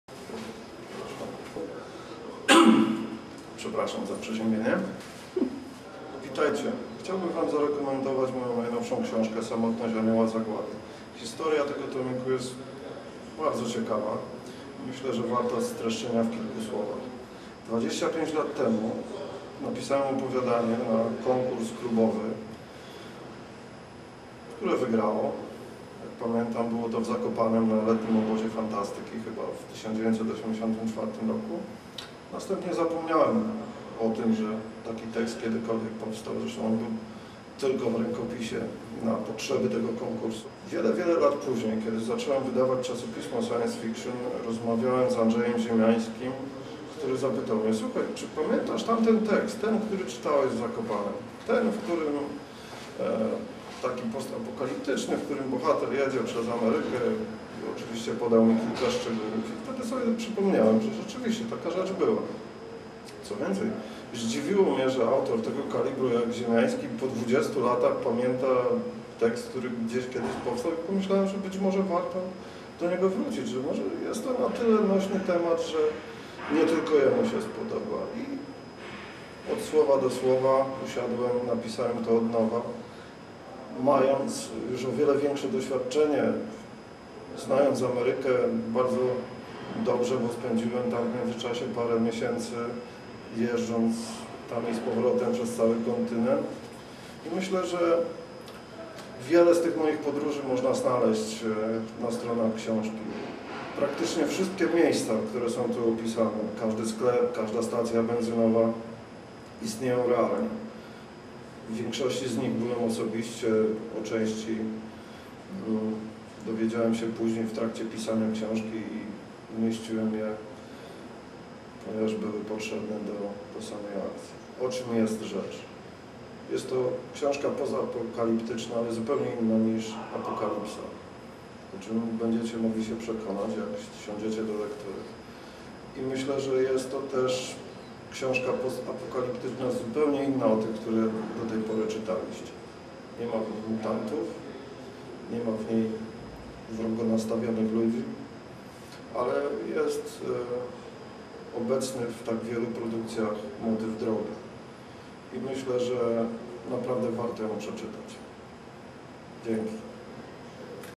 Samotność Anioła Zagłady - o książce opowiada Robert J. Szmidt
Powieść drogi. Rajd przez wyludnioną Amerykę. Podróż wgłąb każdego z nas.
NIE TYLKO DLA FANÓW "FALLOUTA".
Najnowsza książka Roberta J. Szmidta "Samotność Anioła Zagłady" - premiera już 20 listopada!